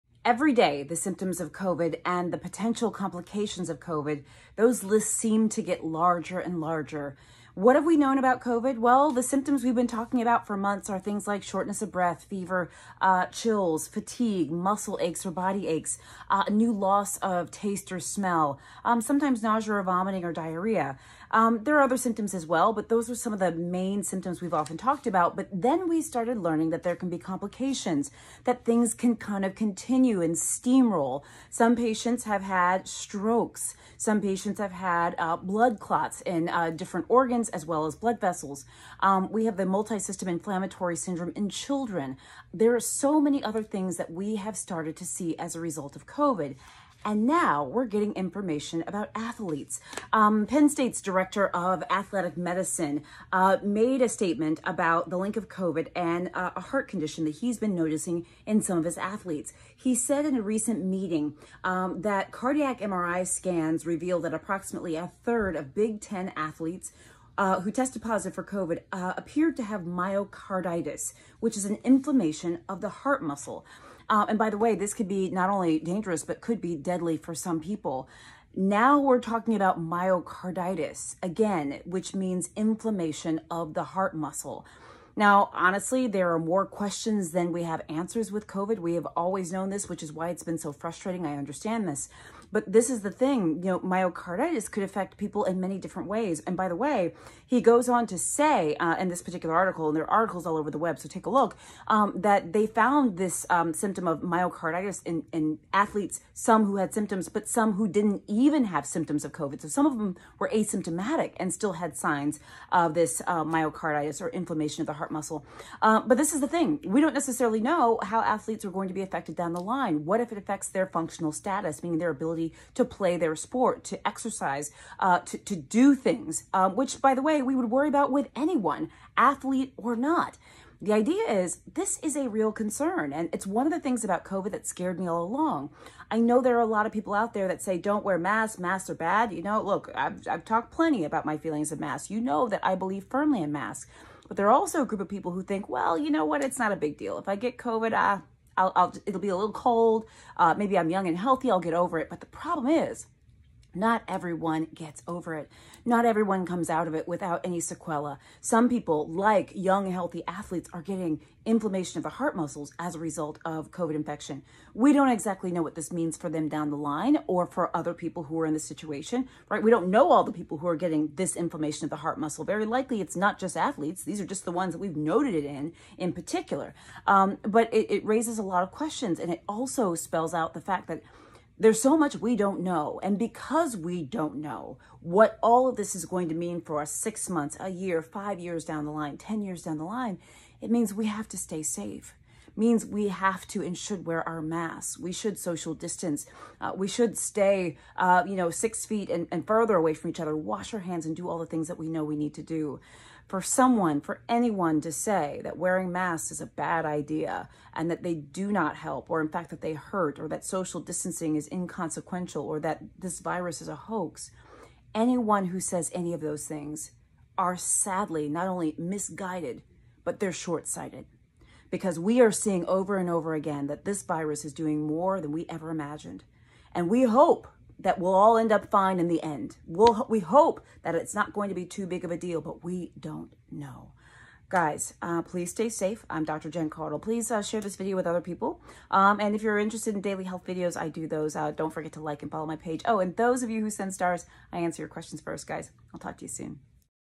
1/3 of Big Ten Athletes with COVID Found to Have Heart Inflammation (Myocarditis). A Doctor Explains
As time goes on we learn more and more about COVID. The latest involves Big Ten athletes. Take a peek.